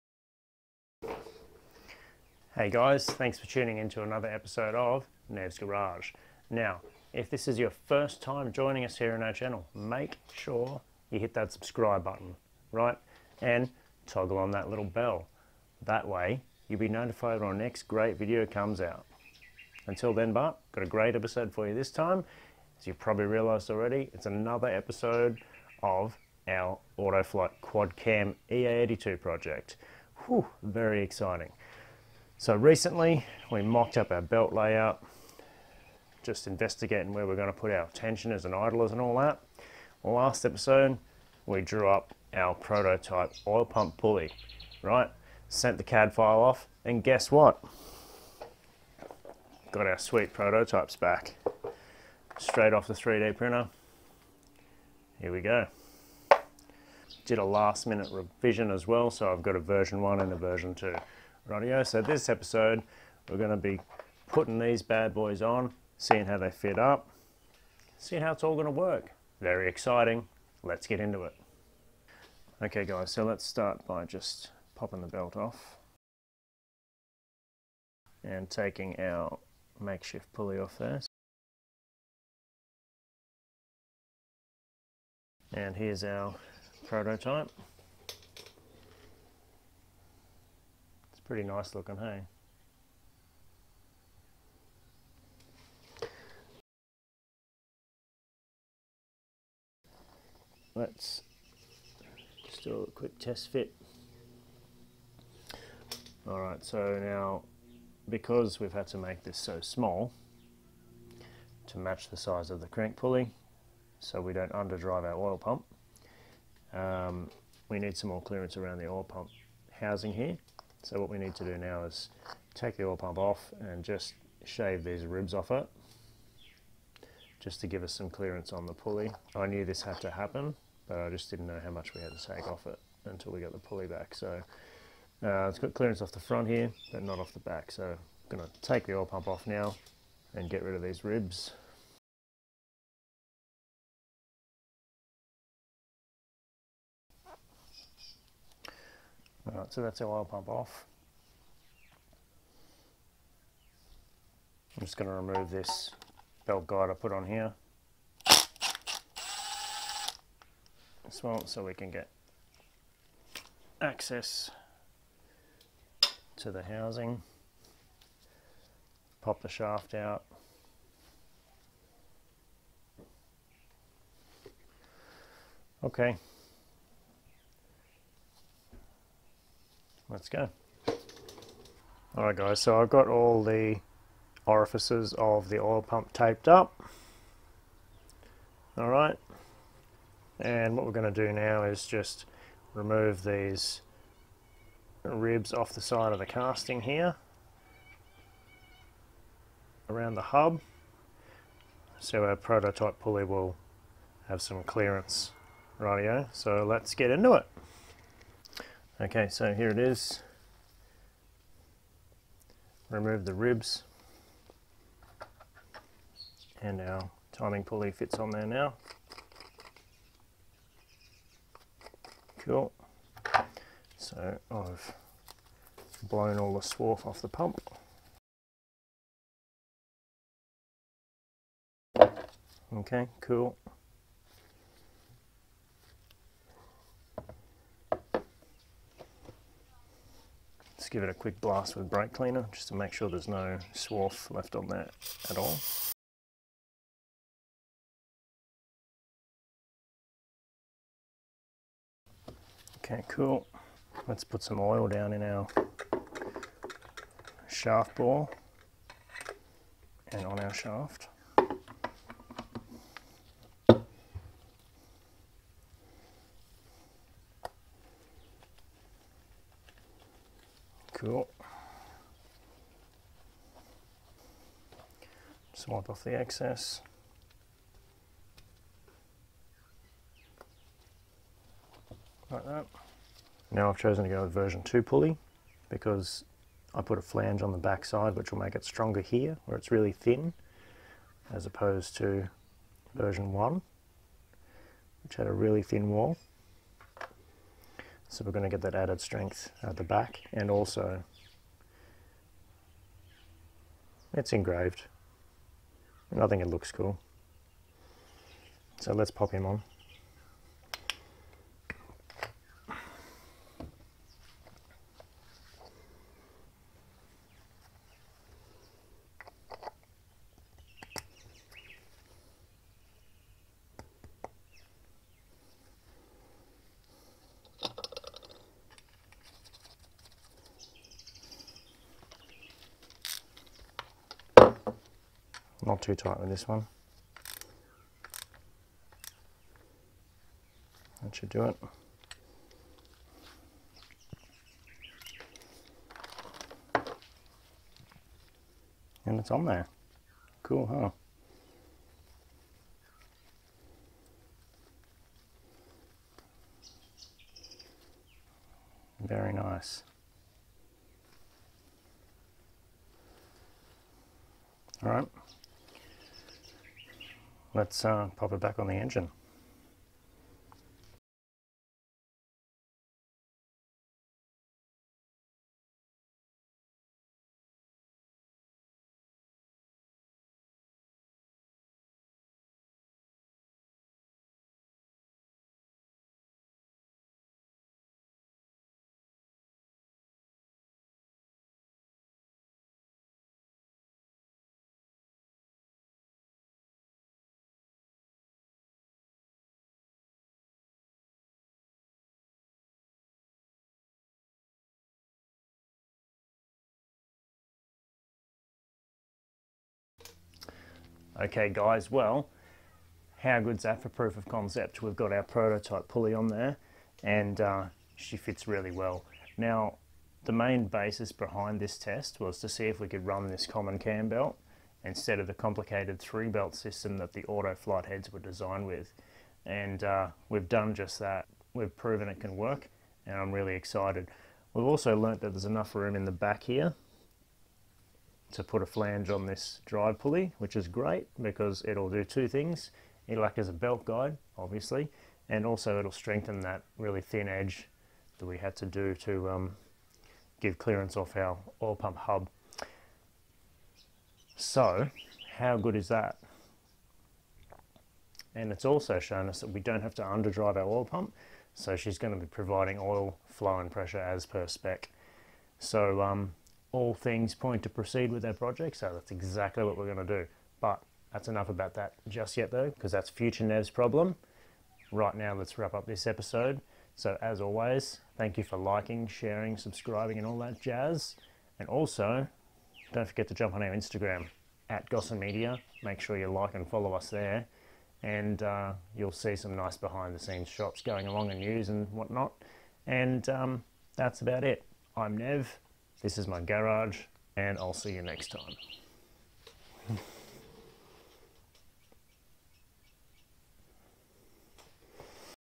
DOHC EA82 Project - Episode 9 : Trial fitting the Prototype Oil Pump Pulley
In this episode we test fit our prototype drive pulley for the ER27 Oil Pump and see how the belt routing looks.